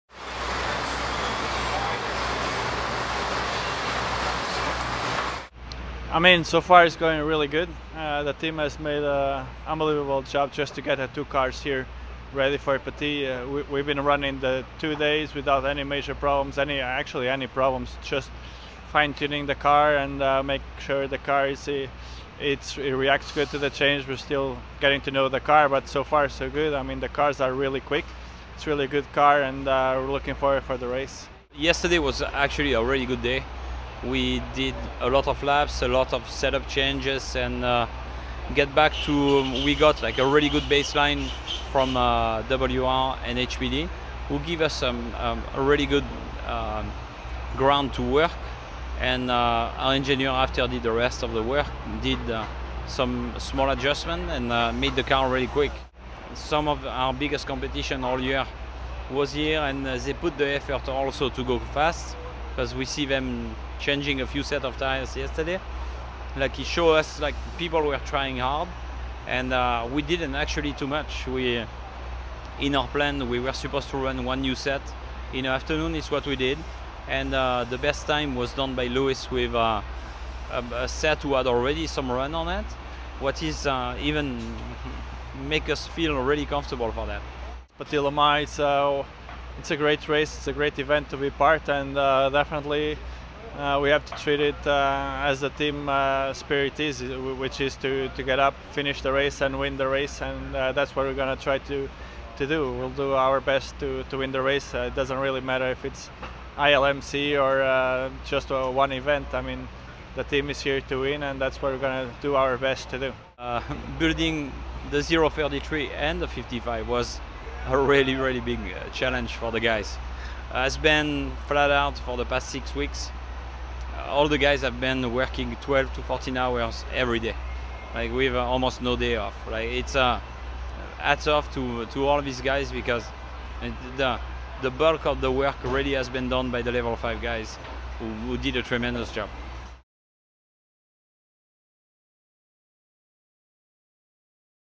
Level 5 Petit Le Mans Tuesday vBlog
Level 5 Motorsports' Joao Barbosa and technical director Jean Marchioni check in from Road Atlanta prior to Wednesday's opening day of official track action for the 14th annual Petit Le Mans powered by Mazda